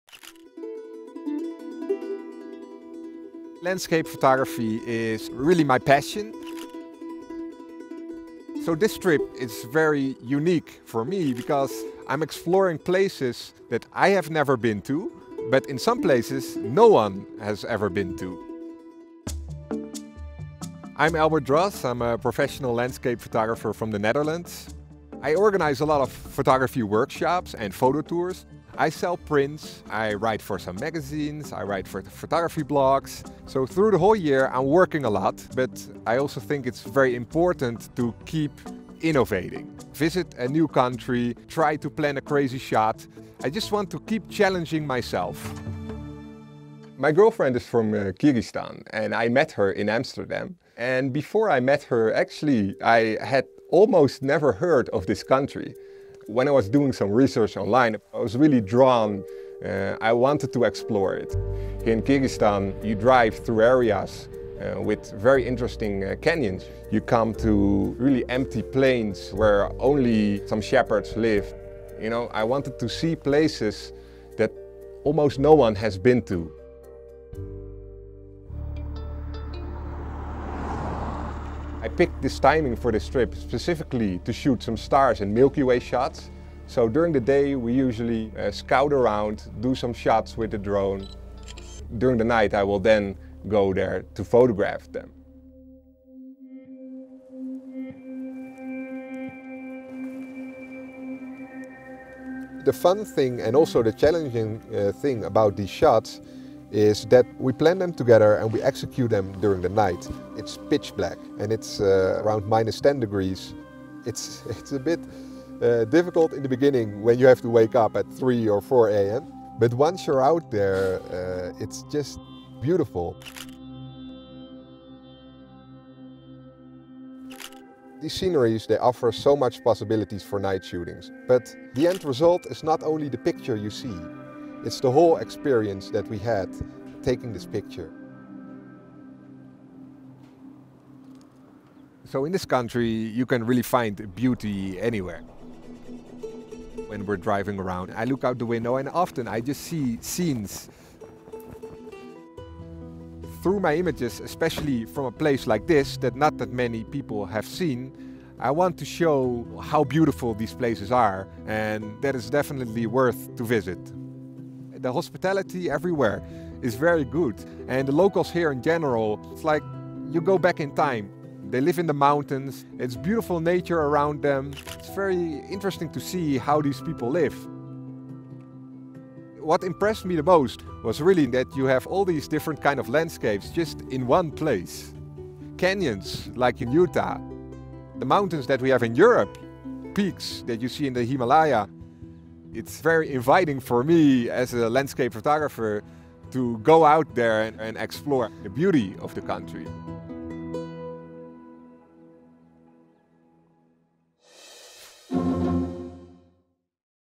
كيف تبدو السماء في قرغيزستان؟
ألبرت دروس، مصور هولندي للمناظر الطبيعية، يتجول في أودية قيرغيزستان المنطقة الأقل شهرة في العالم والنائية في رحلة استكشافية.